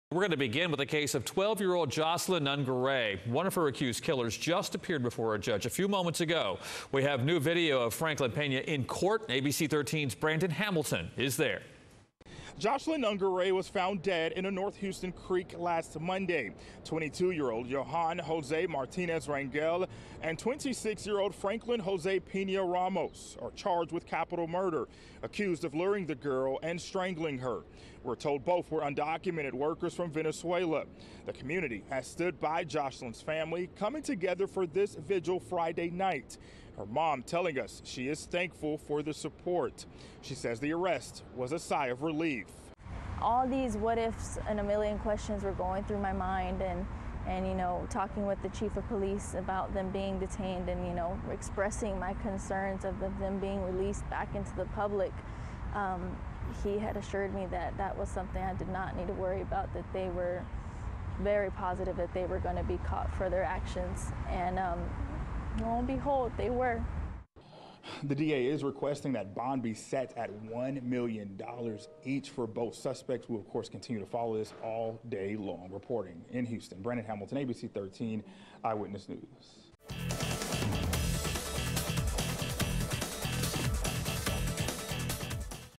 Judge sets $10M bail for 1 of the capital murder suspects accused of strangling 12-year-old girl
Franklin Jose Pena Ramos is believed to be a flight risk, according to the judge. He and Johan Jose Martinez Rangel are accused of strangling Jocelyn Nungaray, then dumping her body in a creek.